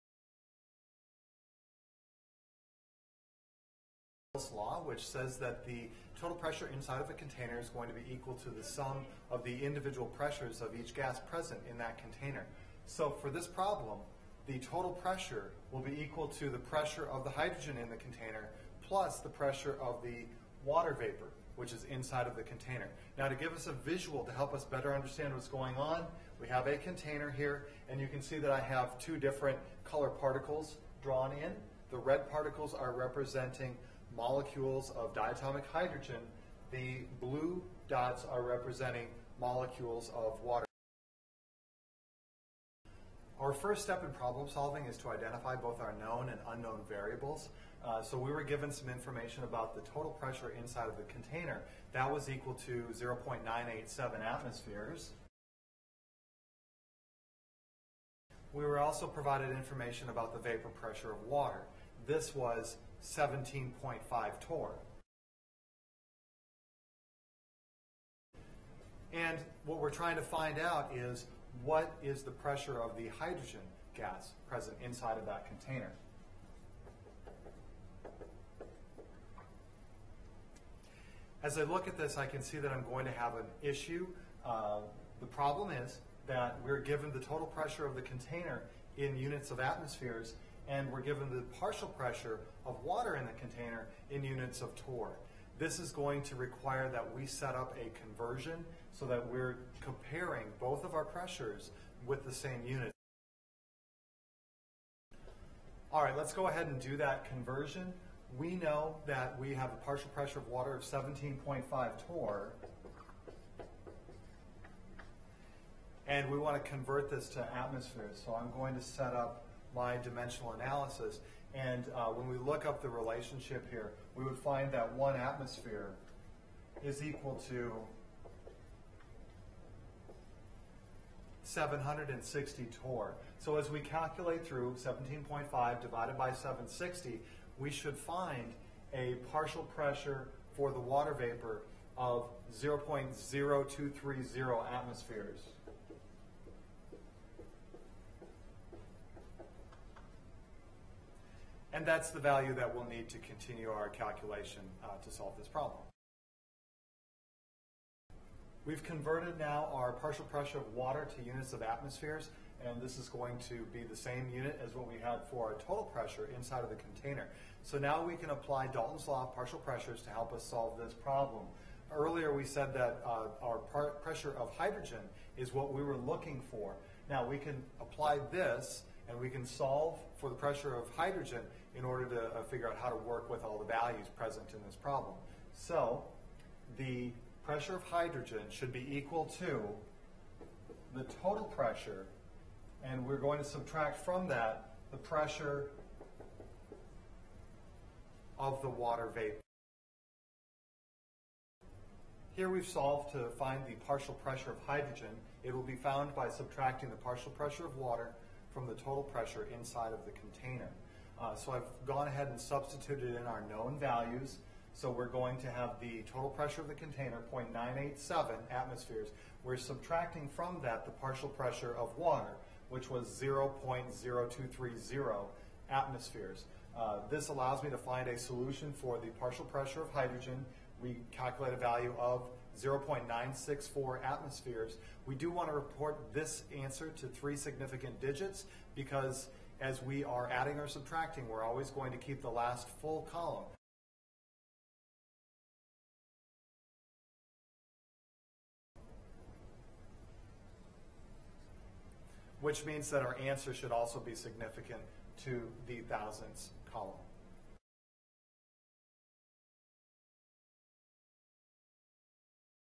How to Complete a Dalton's Law Gas Problem - Mr Pauller
This video illustrates how to calculate the partial pressure of a gas using Dalton's Law of Partial Pressure